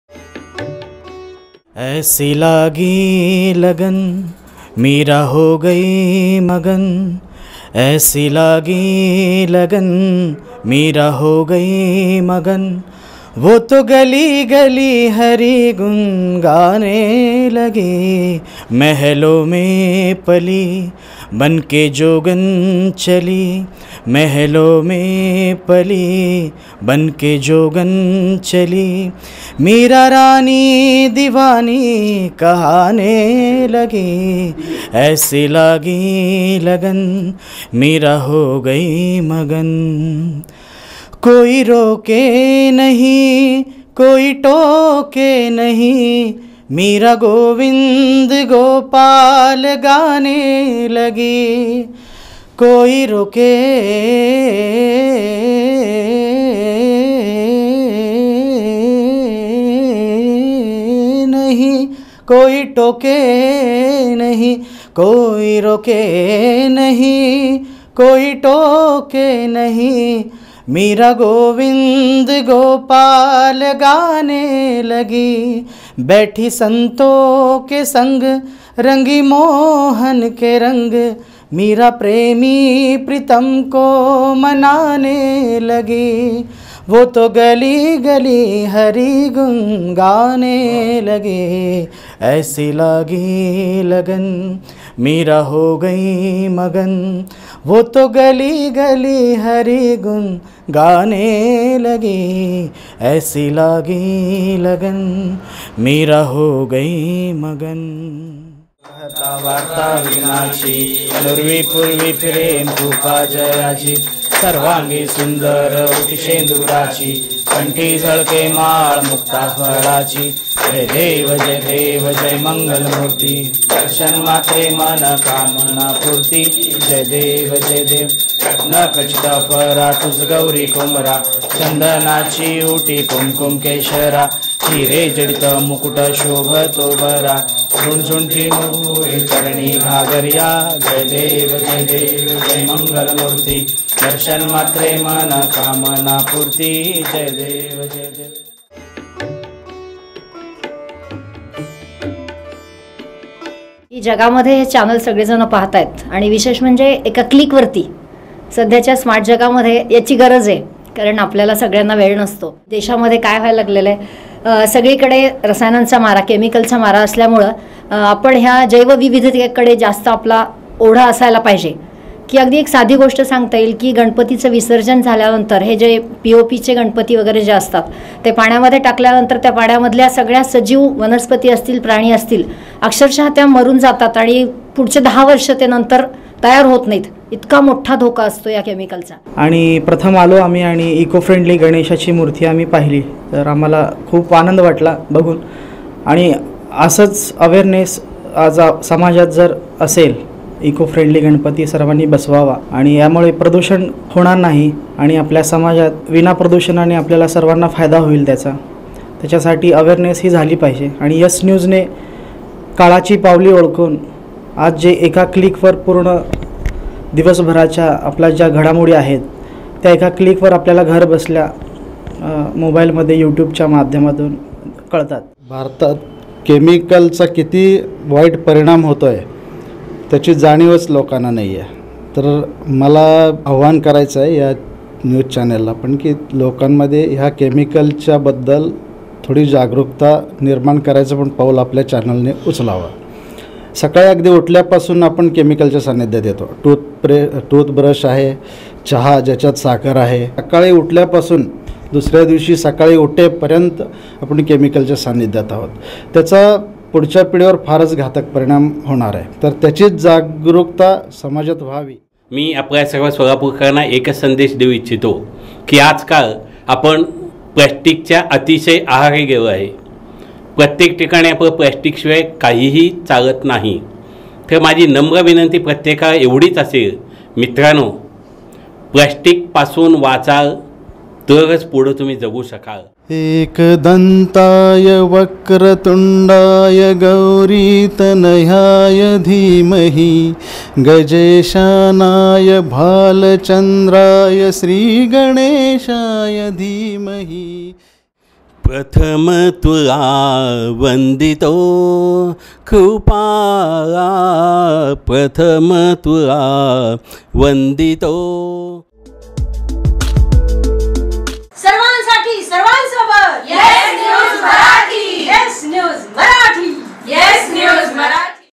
कलावंतांचा गणराया चरणी स्वरसाज
सोलापूर : येस न्युज मराठीच्या इकोफ्रेंडली गणपतीसमोर गुरूवारी सकाळी कलावंतांनी स्वरसाज चढवला. डॉ.सुरेश खमितकर, डॉ.दीपक पोतदार, शिक्षिका अस्मिता पोतदार, गायक व संगीत शिक्षक नागेश पवार आणि नकलाकार अशोक सुरतगावकर यांनी विविध कला सादर करून गणराया चरणी आपली सेवा रूजू केली.